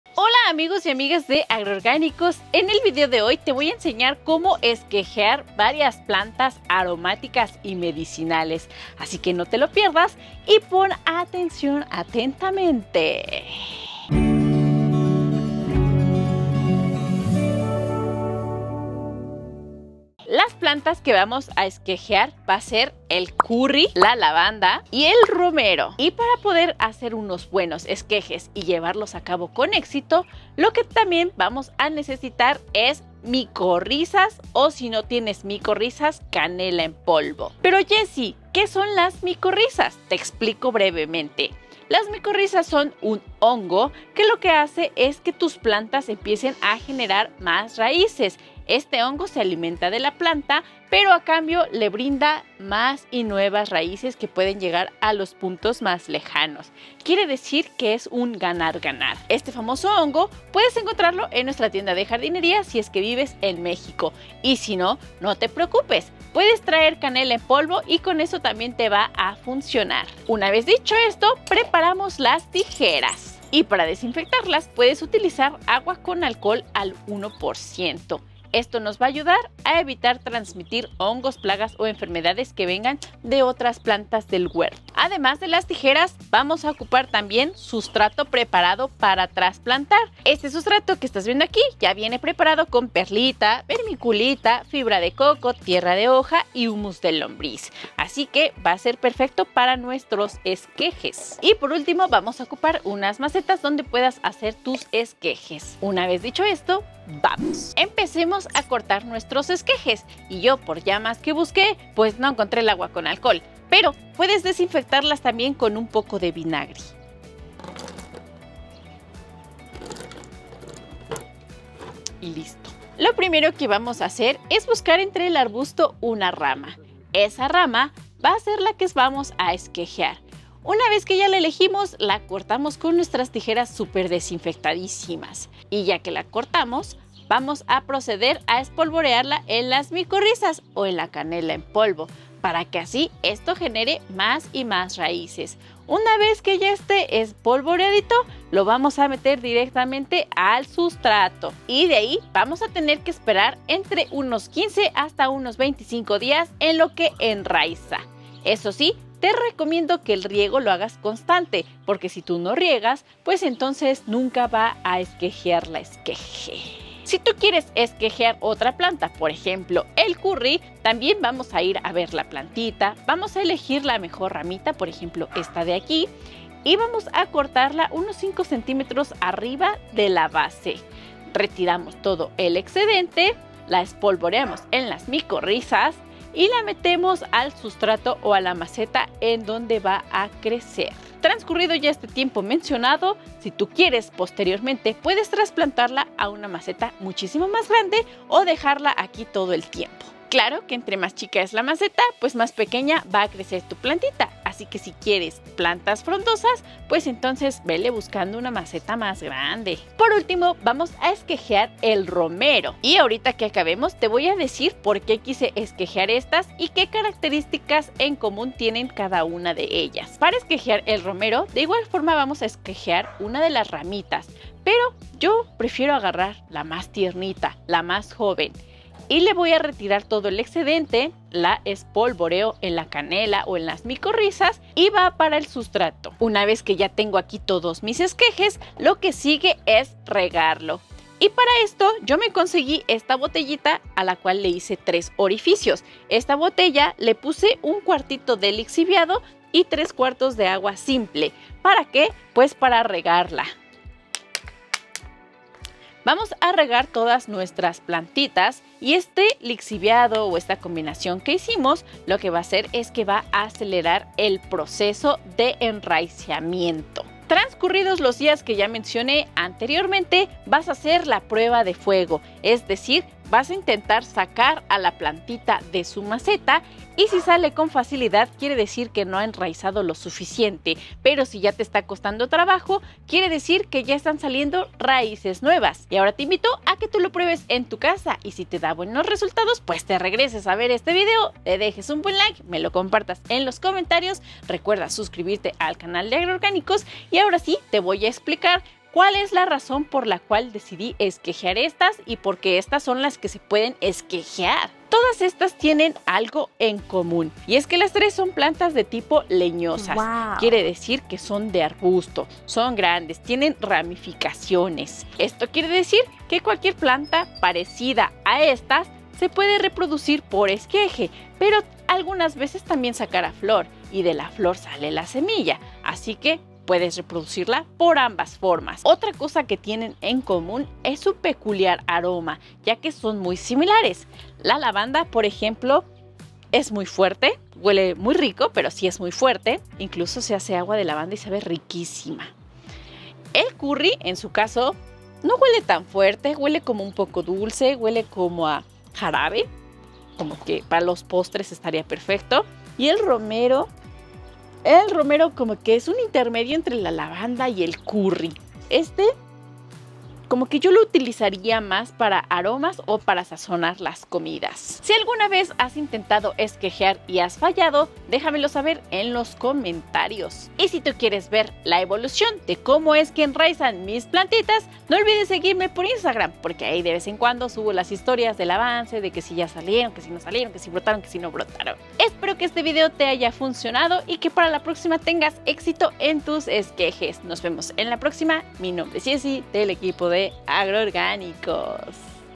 🤩 COMO HACER ESQUEJES de tus PLANTAS! 🌱 ¿Y que plantas se pueden esquejar?
🔥 Hoy te enseño unos trucos para poder sacar esquejes de forma exitosa!

🪴 Tienda en Amazon

✅ Haz tu pedido por WhatsApp al: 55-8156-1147